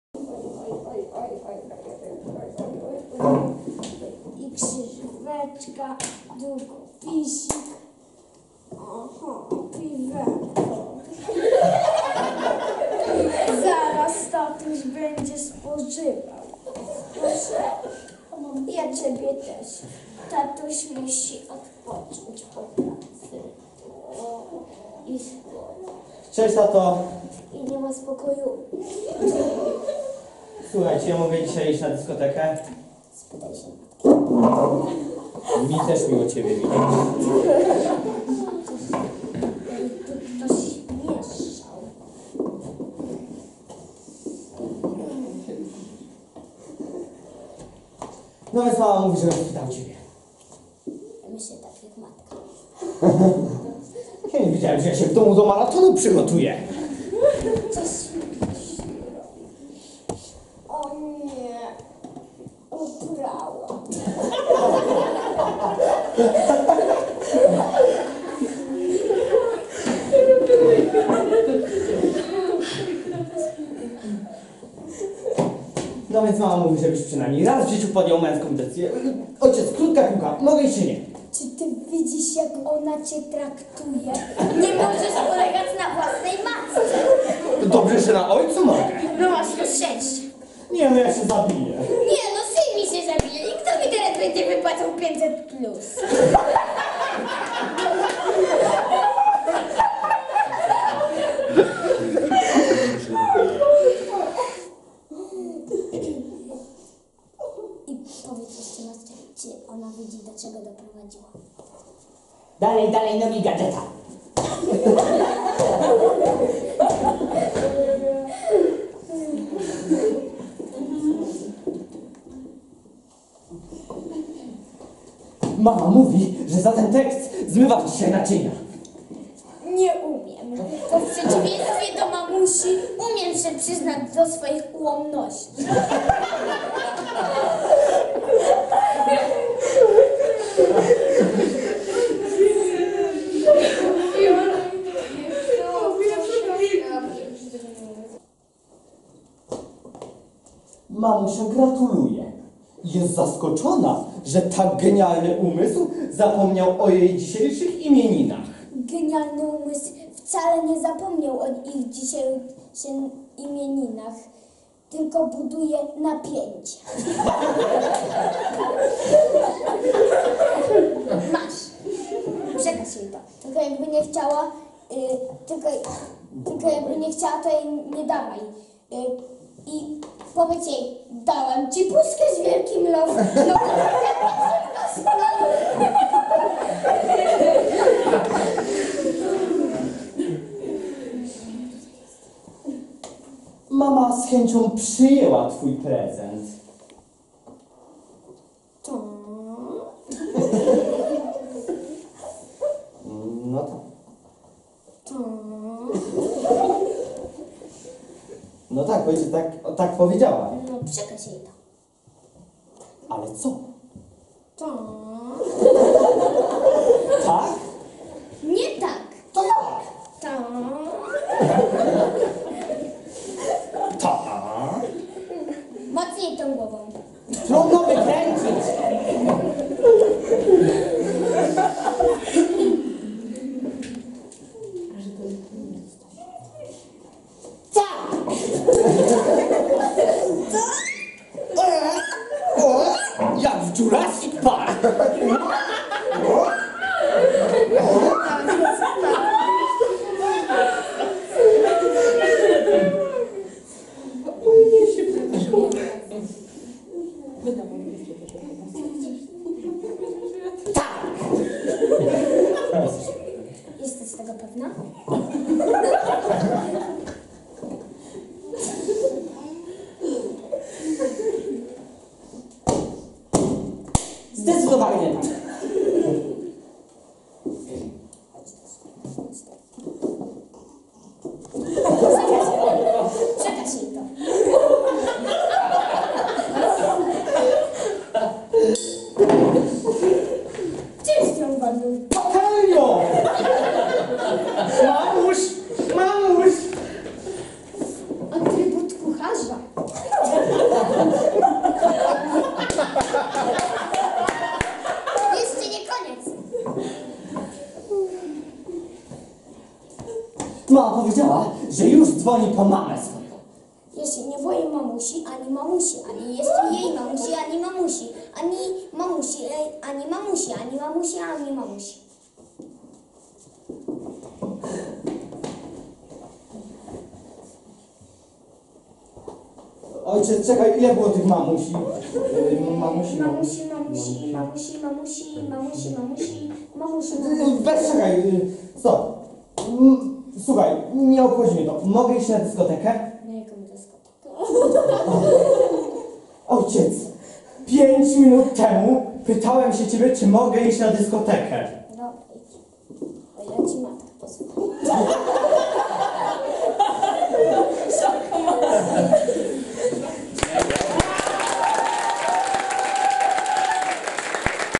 LOL Kościelisko 2019 - Skecz
Liga Obozu Liturgicznego Kościelisko 2019 - obóz ministrantów, ministrantek oraz scholii z parafii pw Św. Andrzeja Boboli w Bukownie.